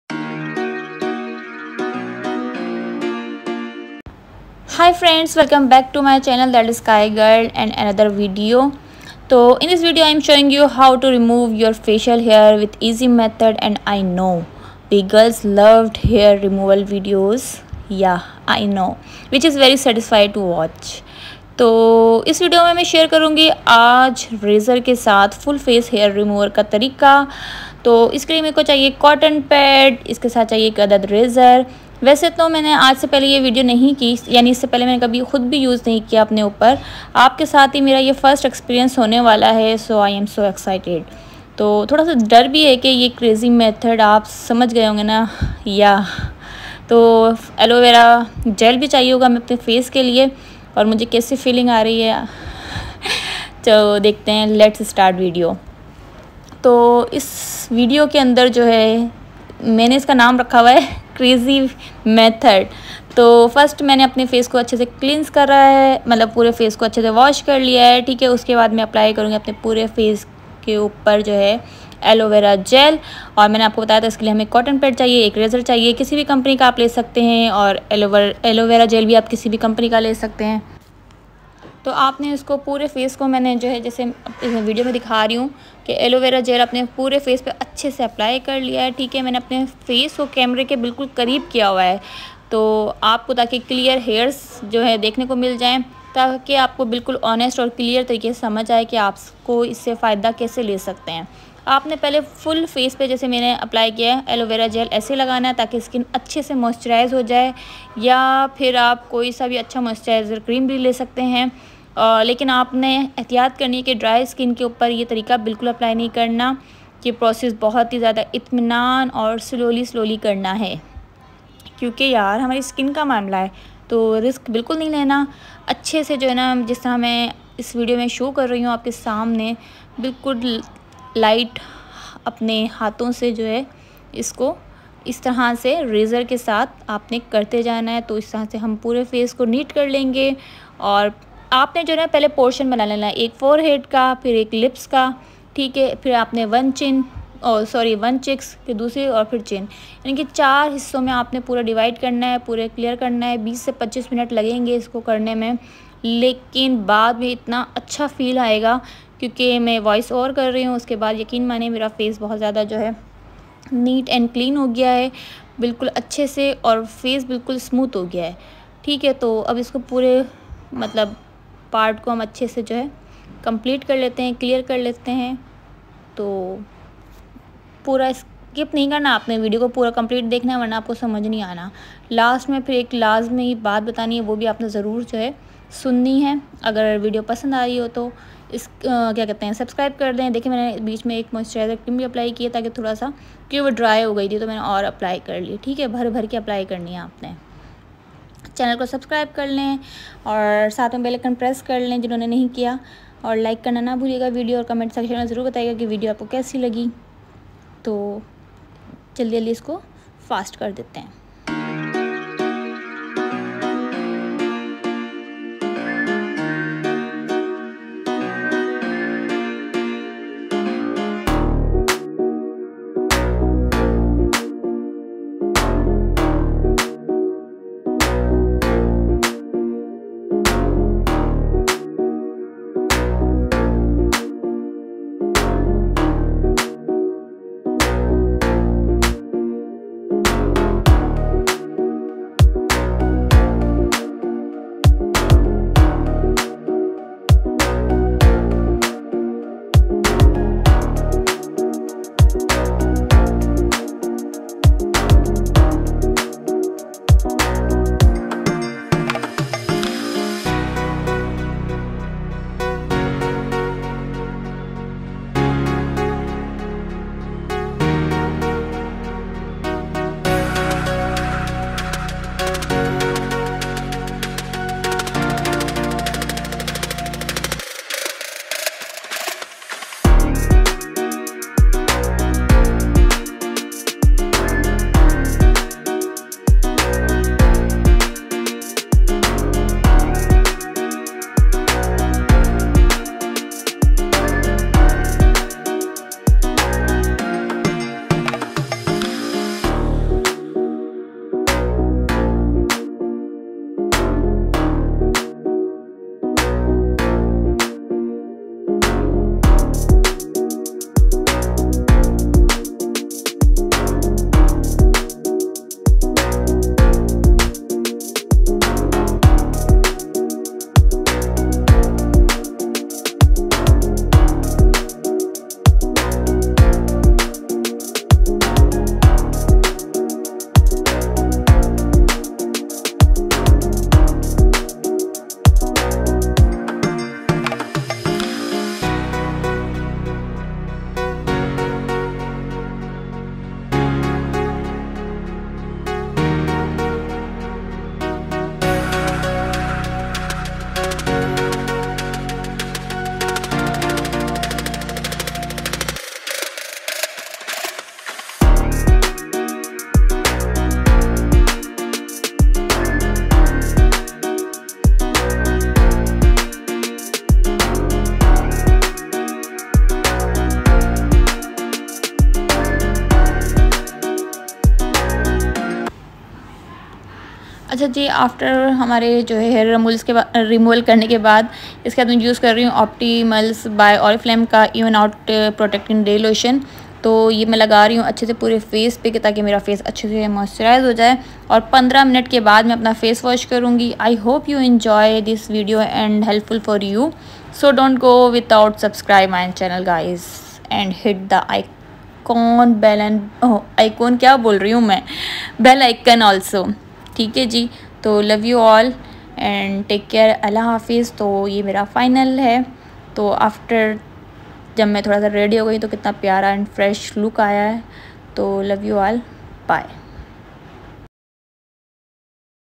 PAINLESS Hair Removal method || How to Remove Facial Hair at home || that sky girl & vlogs
Remove Facial Hair at Home Naturally, No Waxing No Threading - Tips & Misconceptions